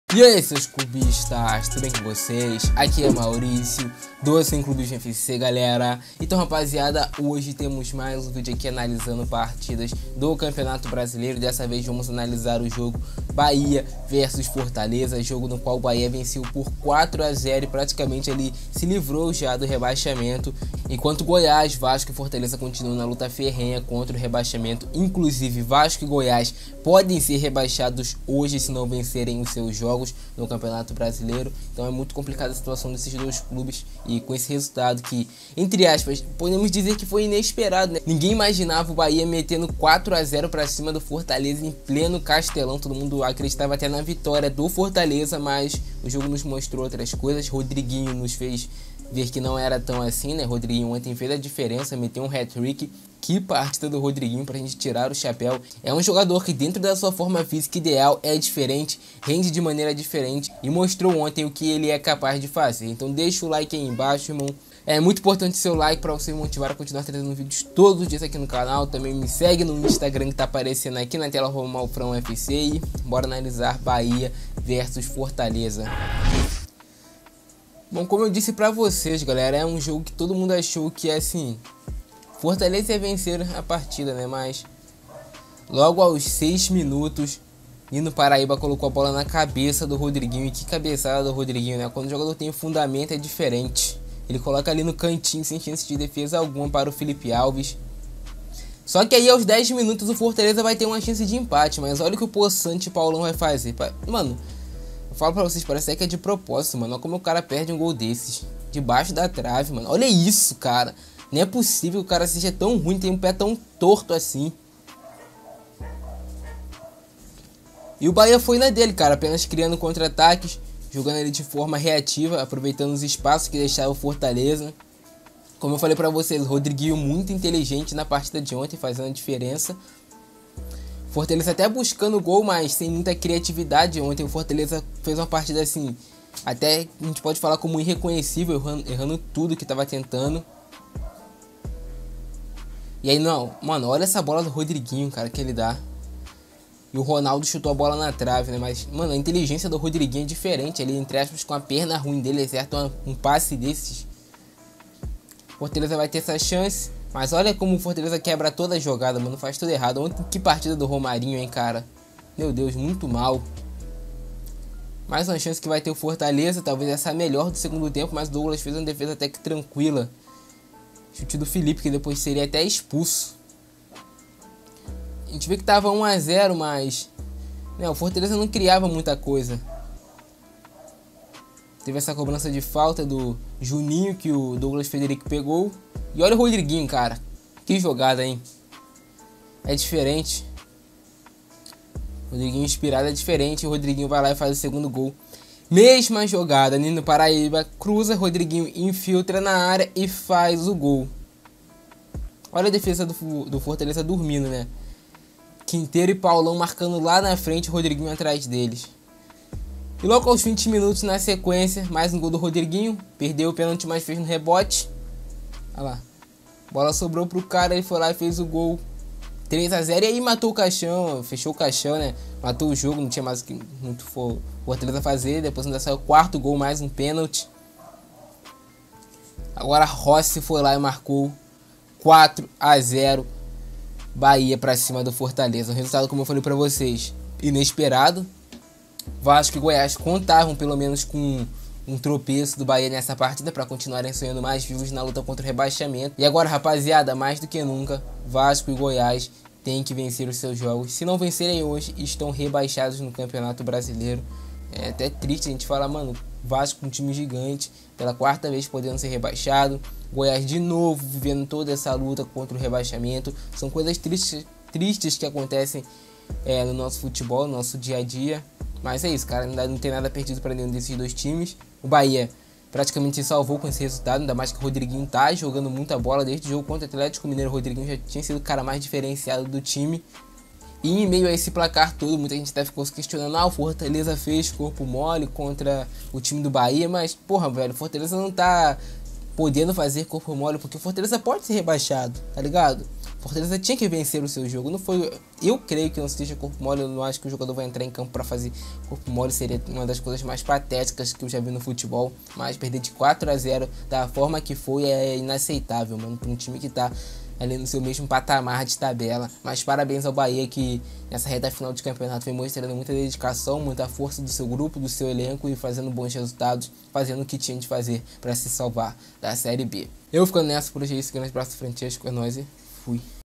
BAHIA ATROPELA O FORTALEZA!! VASCO PRECISA DE MILAGRE EM ITAQUERA - ANALISE FORTALEZA X BAHIA
Então galera hoje temais mais um video aqui no canal analisando jogos do brasileirão 2020, dessa vez vamos analisar bahia x fortaleza, bahia venceu o fortaleza por 4x0 no castelão e deixou o vasco precisando de um milagre pra se livrar do rebaixamento hoje contra o corinthians.

LOJA